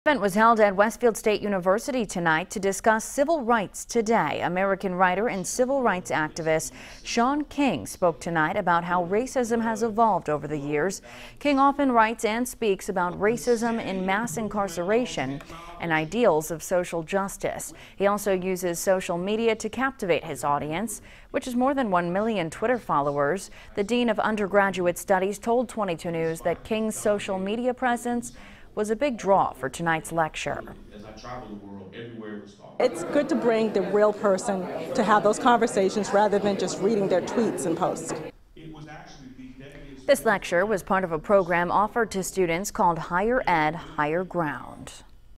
American writer, civil rights activist Shaun King speaks at Westfield State
American writer and civil rights activist Shaun King spoke Monday night about how racism has evolved over the years.